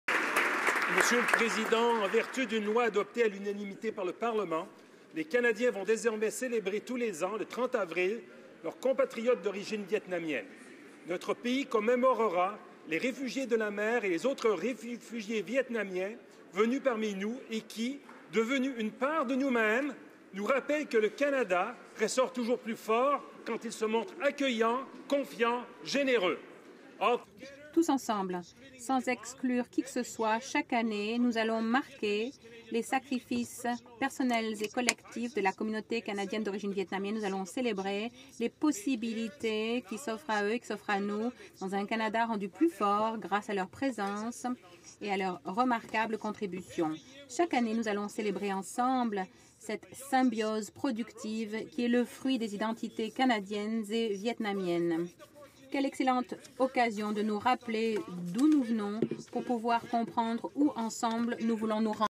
Déclaration en Chambre: La Journée du parcours vers la liberté
En vertu d'une loi adoptée à l’unanimité par le Parlement, les Canadiens vont désormais célébrer tous les ans, le 30 avril, leurs compatriotes d'origine vietnamienne. Voici ma déclaration que j'ai faite à ce sujet en Chambre, le 30 avril 2015.